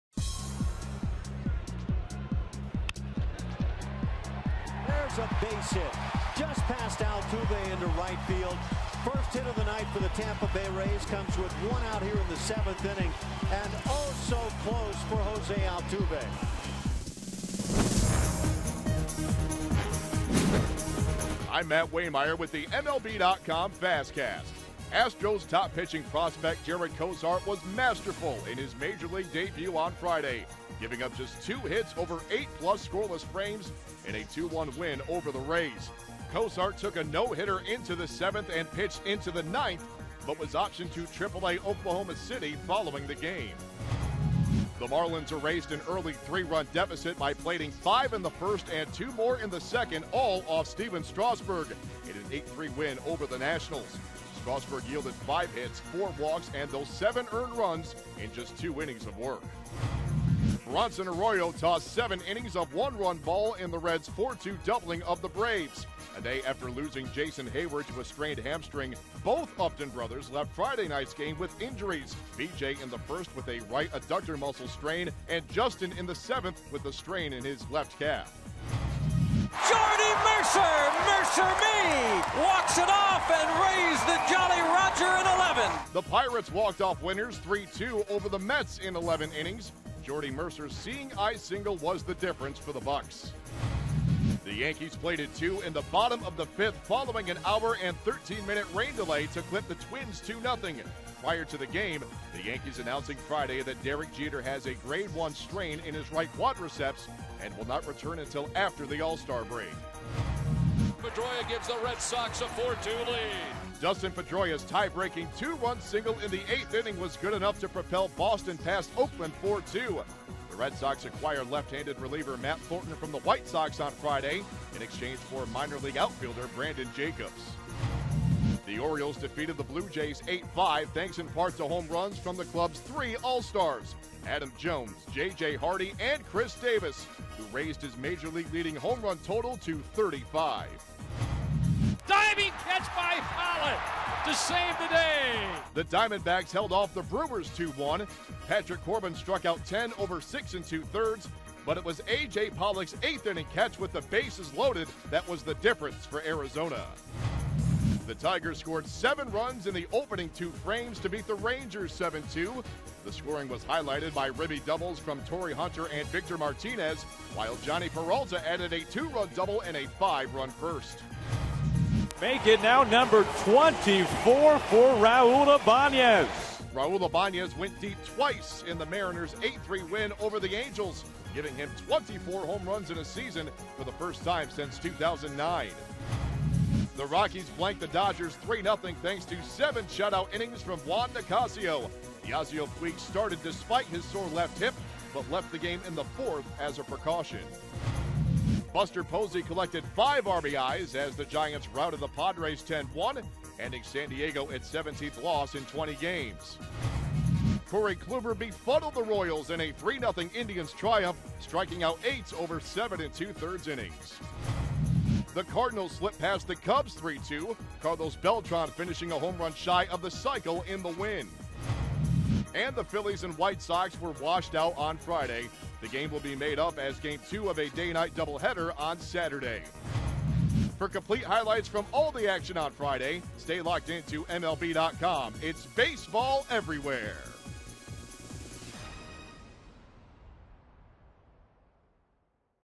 7/12/13 MLB.com FastCast: Cosart dazzles in debut
Jarred Cosart takes a no-hitter into the seventh in his Major League debut, plus the Marlins tag Stephen Strasburg for seven runs